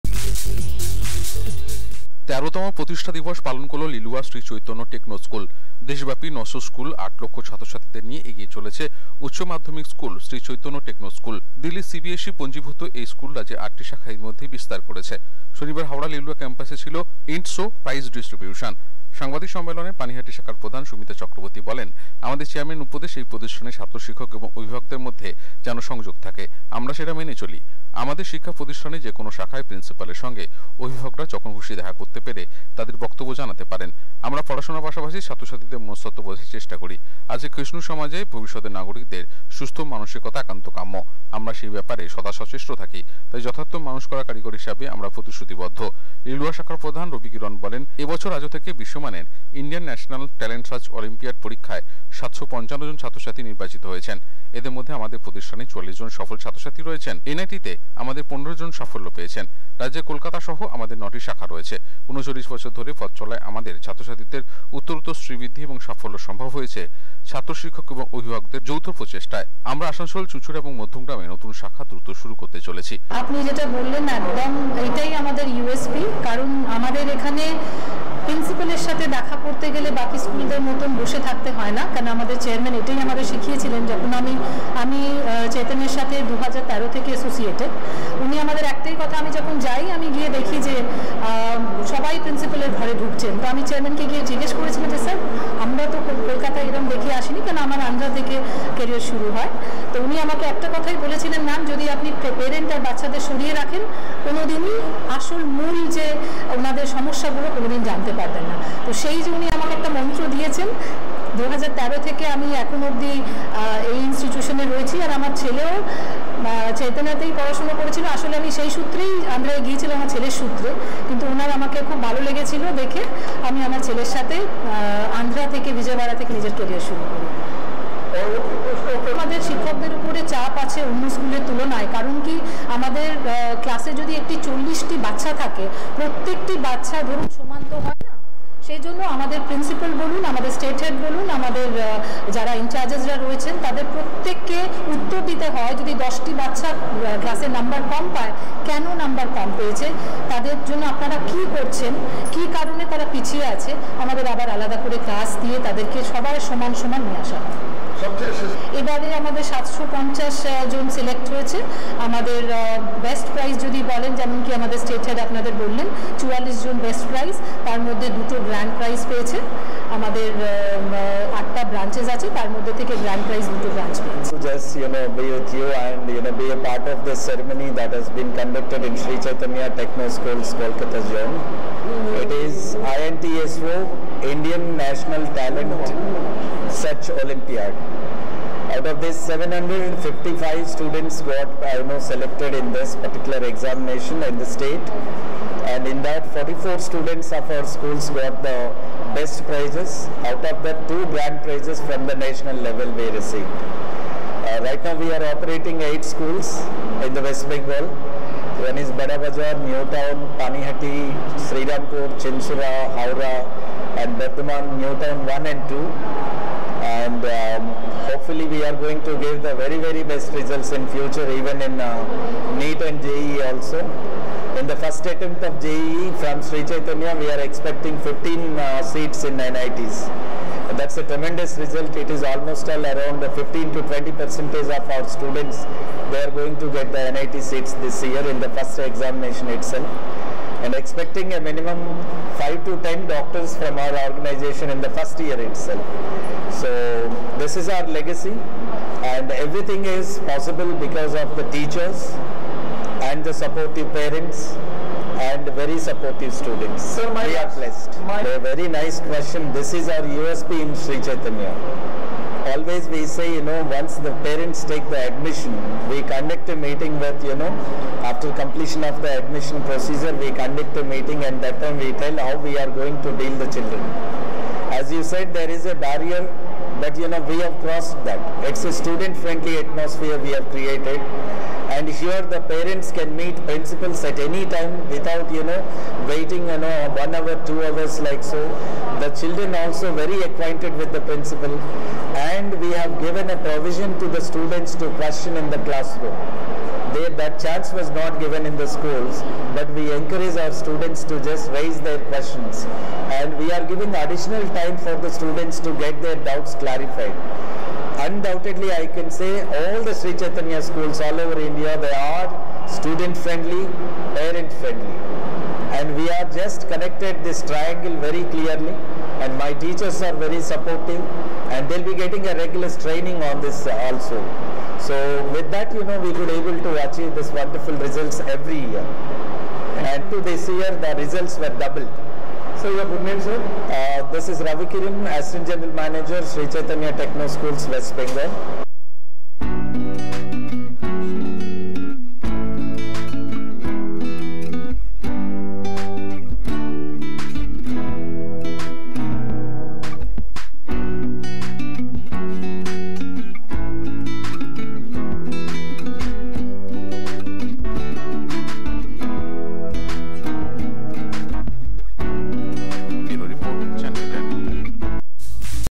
১৩ তম প্রতিষ্ঠা দিবস পালন করল লিলুয়ার শ্রী চৈতন্য টেকনো স্কুল
১৩ তম প্রতিষ্ঠা দিবস পালন করল লিলুয়ার শ্রী চৈতন্য টেকনো স্কুল BanglaNews BengaliNews বাংলাতাজাখবর BengaliNewsLive বাংলাখবর CHANNEL10 CHANNEL 10